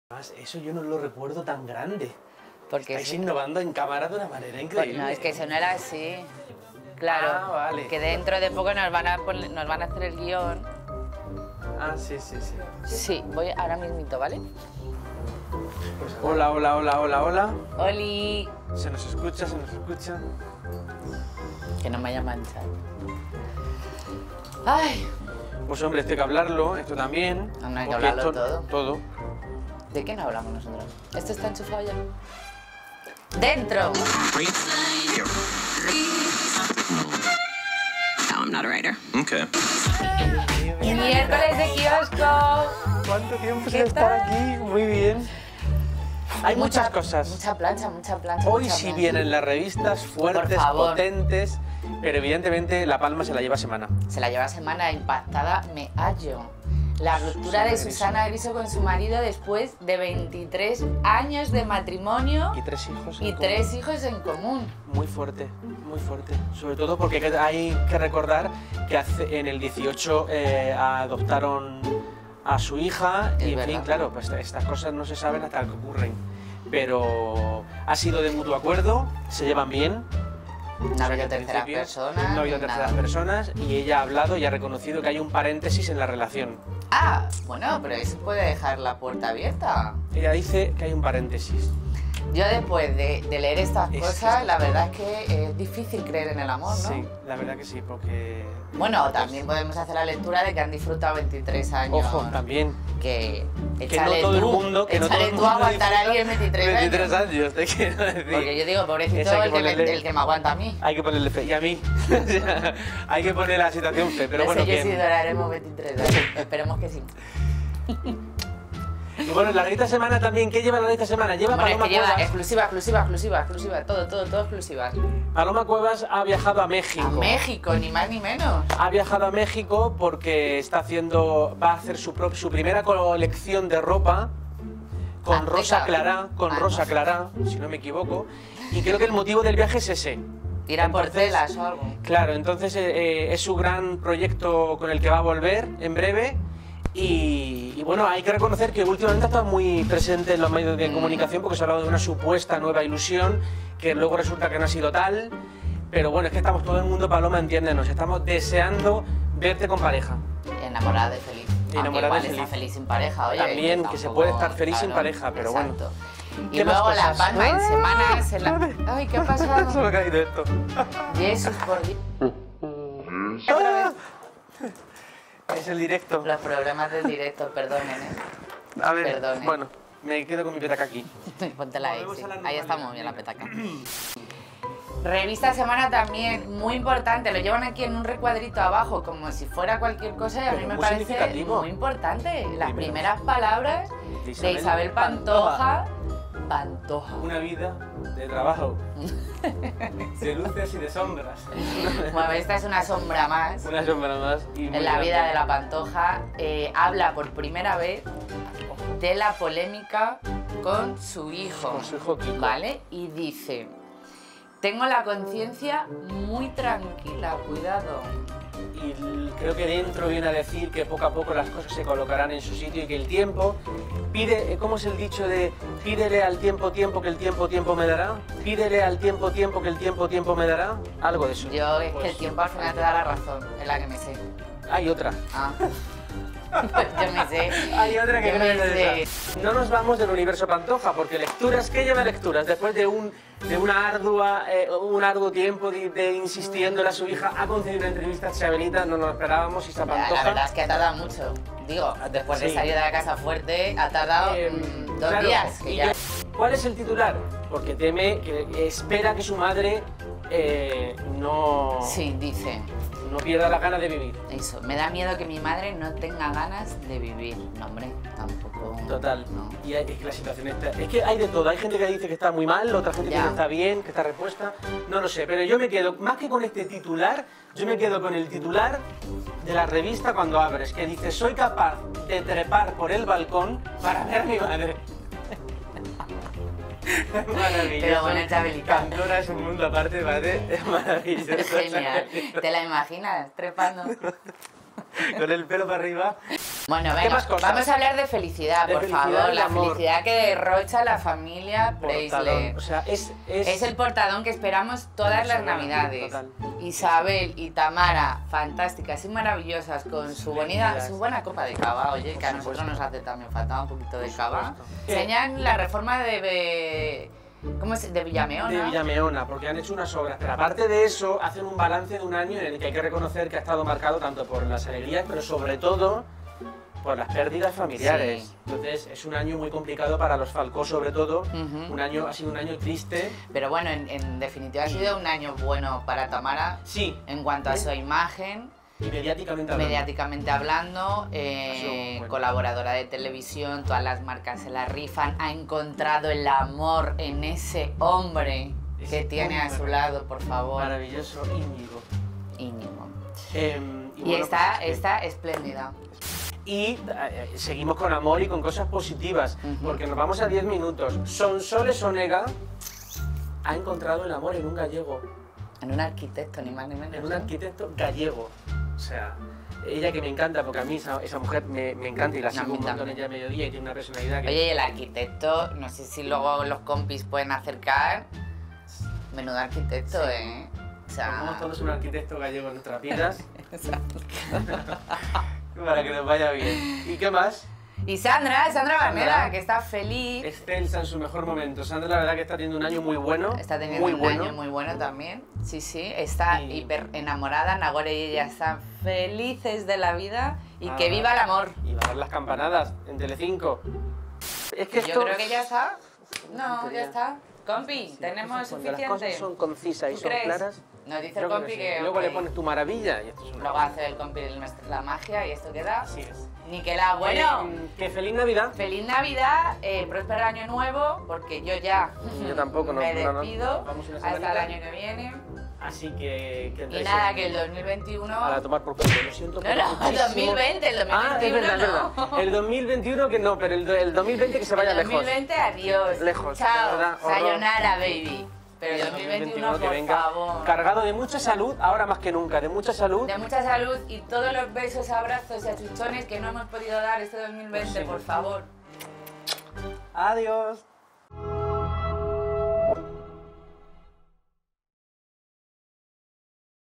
Kiosco rosa: Isa Pantoja confiesa que tiene miedo por el estado anímico de su madre tras la polémica
Miércoles de revistas. Una semana más, JALEOS realiza un repaso exhaustivo de las noticias más destacadas de las publicaciones especializadas en corazón, que como cada miércoles, han llegado a todos los kioscos de España. Por un lado, la revista ¡HOLA! lleva a su portada a la familia Preysler, Isabel Preysler (69 años) y su hija Tamara Falcó (39). Madre e hija enseñan en exclusiva la reforma de su casa tras un año marcado por las tristezas y las alegrías.